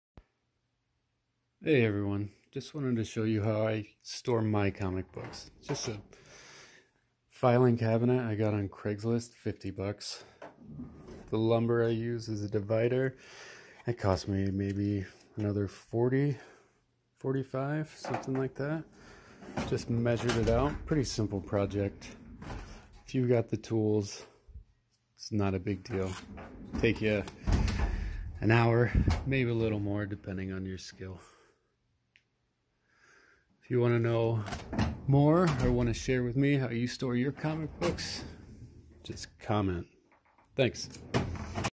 Filing Cabinet for Storing Comic Books
This is how I store some of my comic books.  Easy project, inexpensive,  and I find it a lot easier to manage than having long boxes everywhere.

How do you store yours?